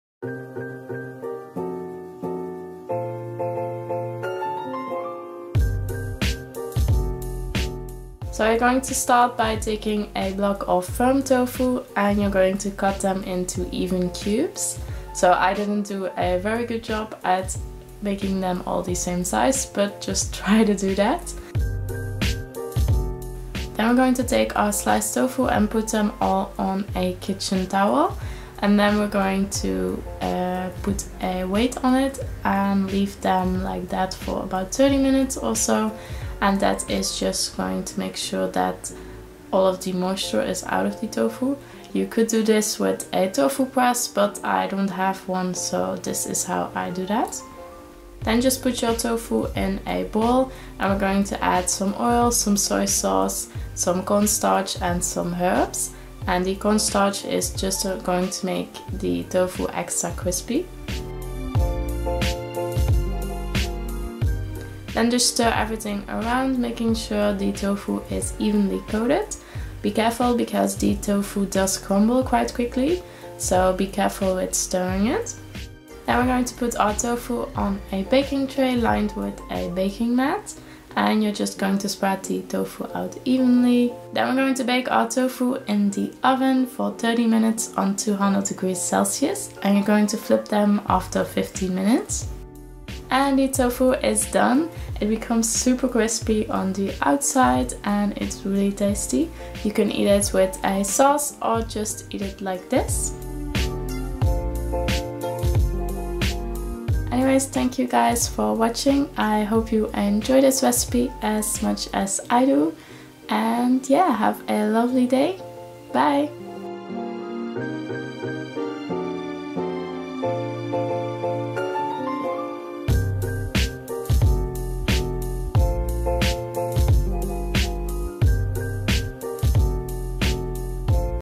CRISPY baked TOFU recipe
Hi guys!

RECIPE

For this recipe you'll need:

500gr firm tofu
1tbsp oil
1tbsp cornstarch
1tbsp soy sauce
1tsp paprika powder
1tsp garlic powder
pinch of salt & pepper
1/2tsp ginger powder

Cut your tofu into even sized cubes and press most of the water out. You can use a tofu press to do this or use a heavy weight and a towel. Put the tofu cubes in a bowl and add the rest of the ingredients. Mix until all the tofu is evenly coated, be careful to not crumble the tofu. Put the coated tofu on a baking tray lined with parchement paper and bake in a preheated oven for 30 minutes on 200 degrees Celsius. Flip the tofu halfway through (after 15min.). Enjoy!

Xx Leonora